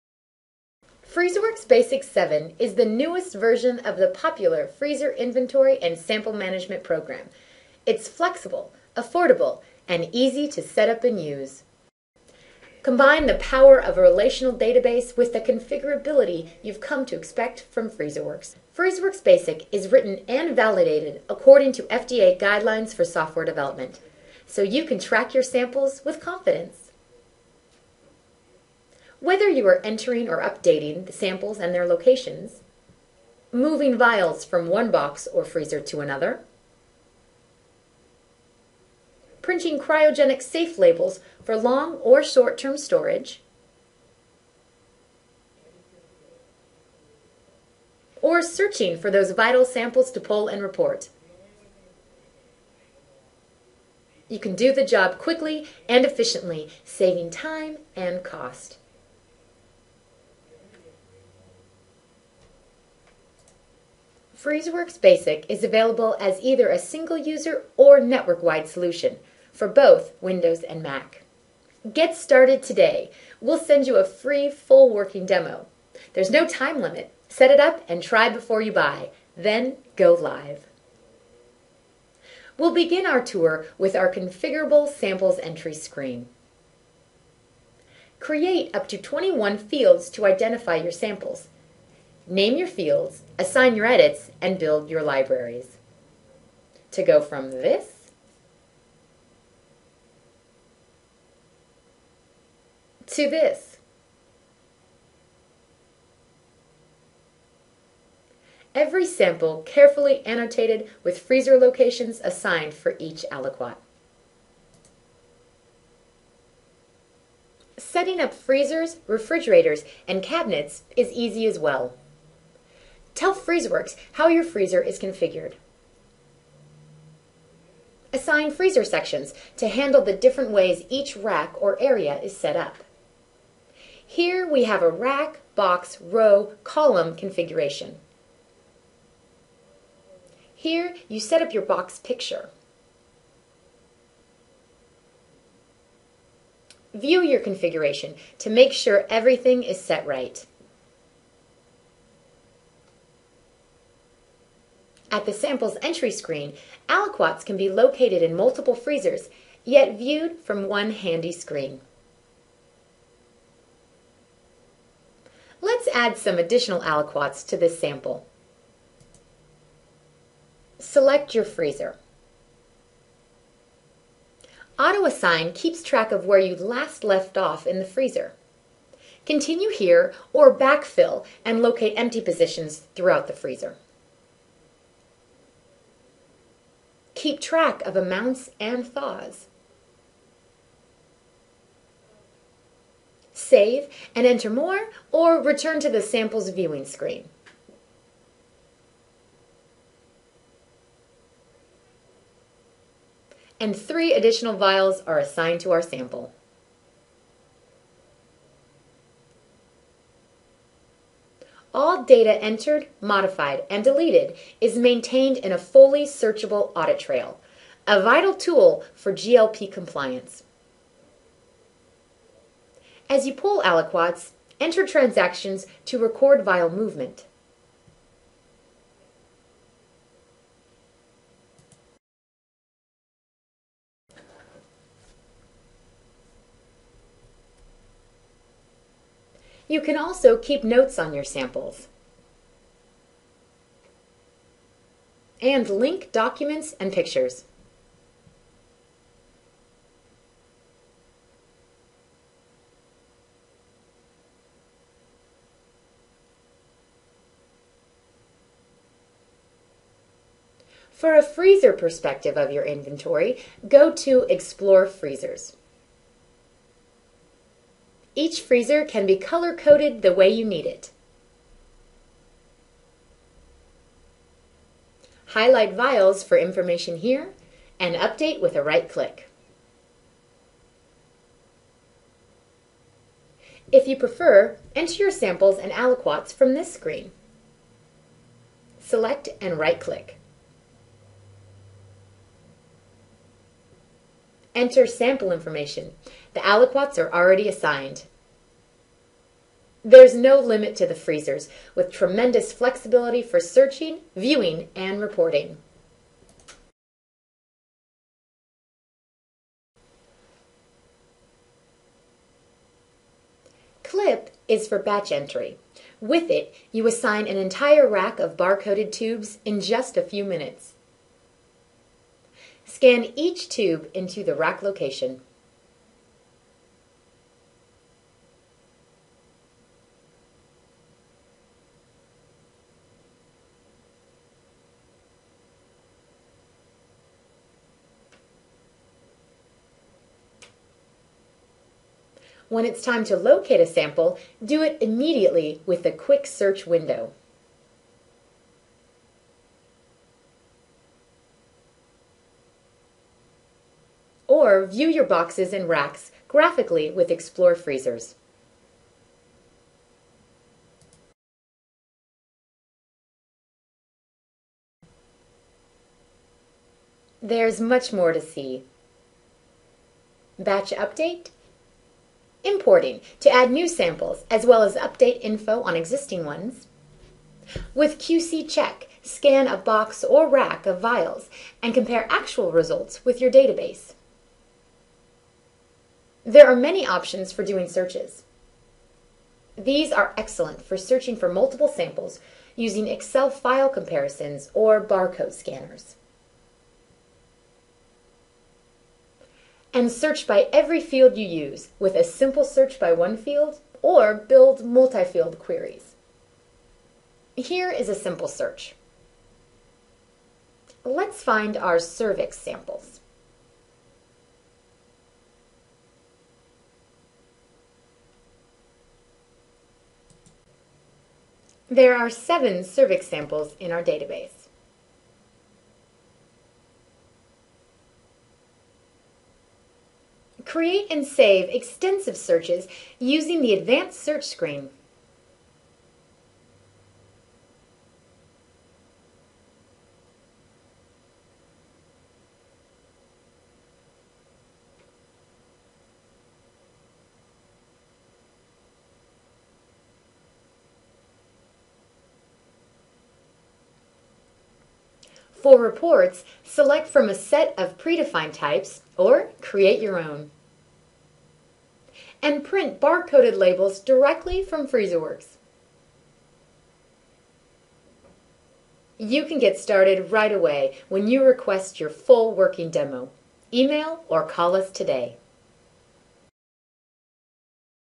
Freezerworks Basic
Freezerworks Basic is a powerful and easy-to-use program for tracking frozen samples. Freezerworks combines the flexibility of a user-definable information management system with the solid foundation of a relational database.